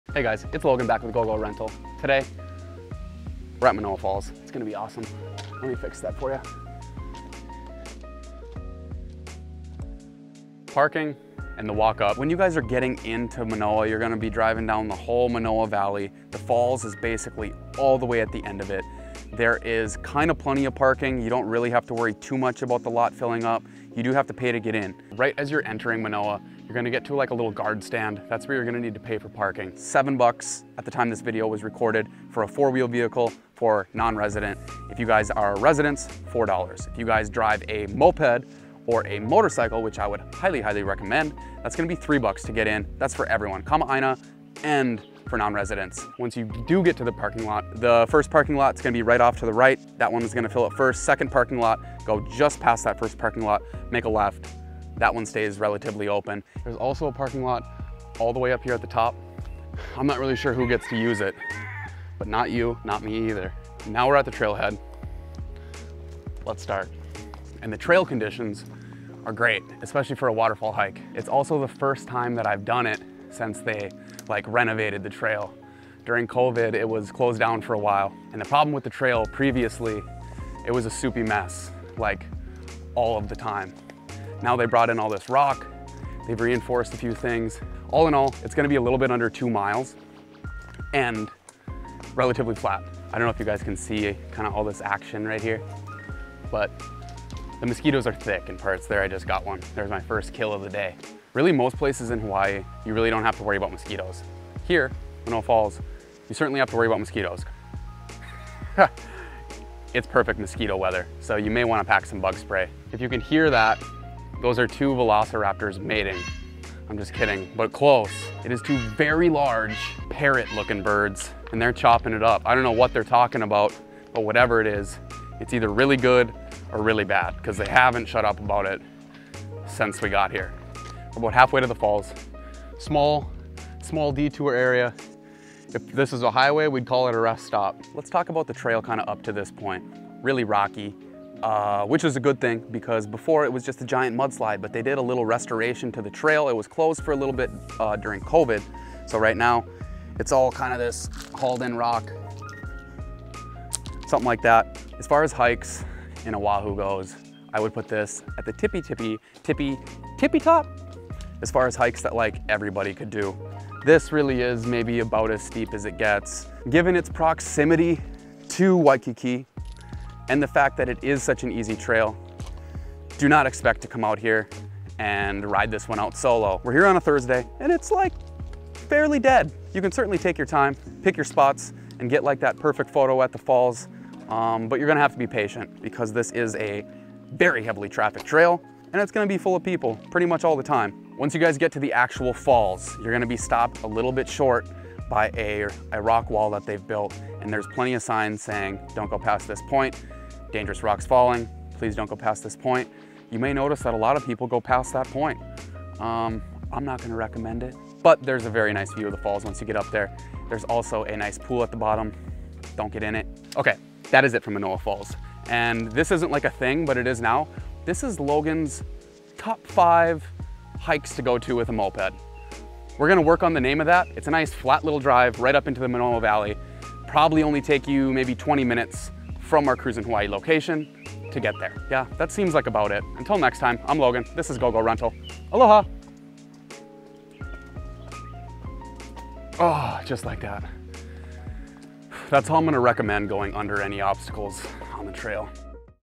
Manoa Falls | Oahu's most popular waterfall hike
Chances are Manoa falls will be your first waterfall hike in Hawaii. The trail head is close to Waikiki, has a decently sized (paid) parking lot. The trail is well maintained and suitable for nearly all people. The waterfall pool is off limits to the public but there is a very nicely constructed viewing area near the base of the falls.

0:00 - Intro
0:10 - Parking and trail access
1:07 - The trail
2:45 - Crowds and the waterfall